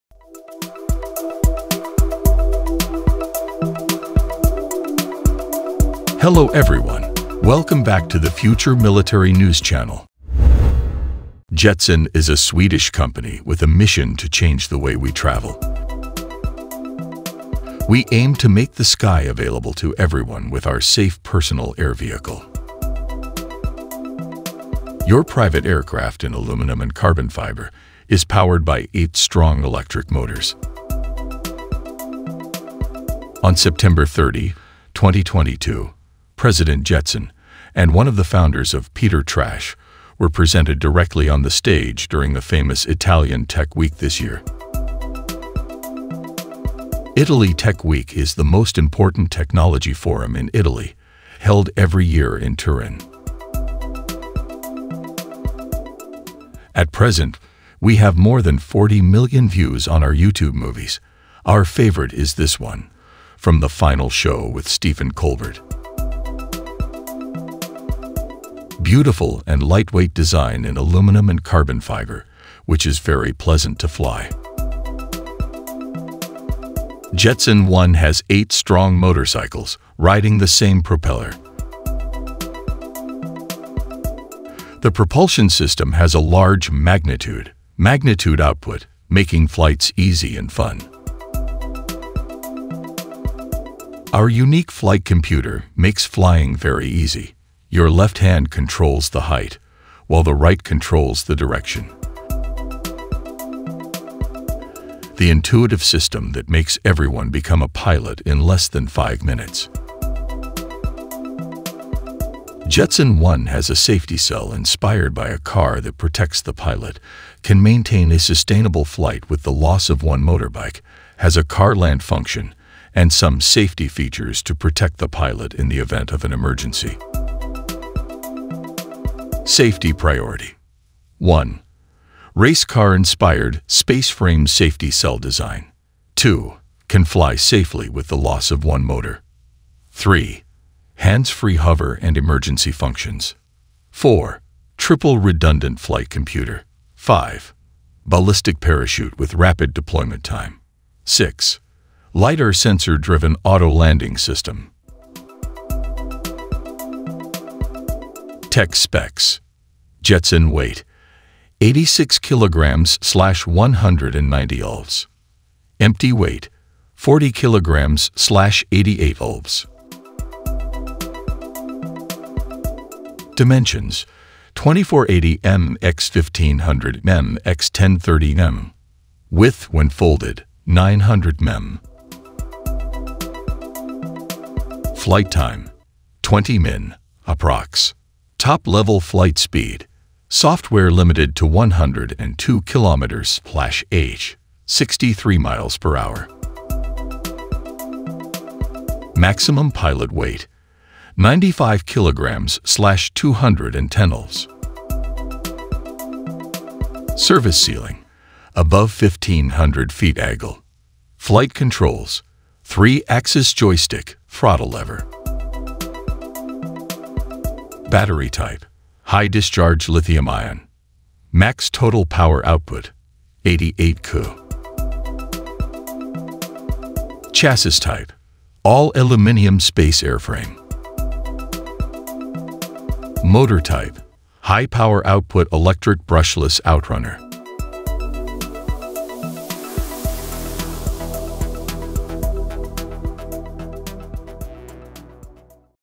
Swedish Aviation Launches Jetson One Drone or Flying Car Can Take Humans to the Sky
Jetson is a Swedish company with a mission to change the way we travel. We aim to make the sky available to everyone with our safe personal air vehicle.

Your private aircraft in aluminum and carbon fiber, is powered by eight strong electric motors.

On September 30, 2022, President Jetson and one of the founders of Peter Trash were presented directly on the stage during the famous Italian Tech Week this year. Italy Tech Week is the most important technology forum in Italy, held every year in Turin.

At present we have more than 40 million views on our Youtube movies. Our favorite is this one, from the final show with Stephen Colbert. Beautiful and lightweight design in aluminum and carbon fiber, which is very pleasant to fly.

Jetson One has eight strong motorcycles, riding the same propeller. The propulsion system has a large -magnitude -magnitude output, making flights easy and fun.

Our unique flight computer makes flying very easy. Your left hand controls the height, while the right controls the direction. The intuitive system that makes everyone become a pilot in less than five minutes.

Jetson One has a safety cell inspired by a car that protects the pilot, can maintain a sustainable flight with the loss of one motorbike, has a car land function, and some safety features to protect the pilot in the event of an emergency.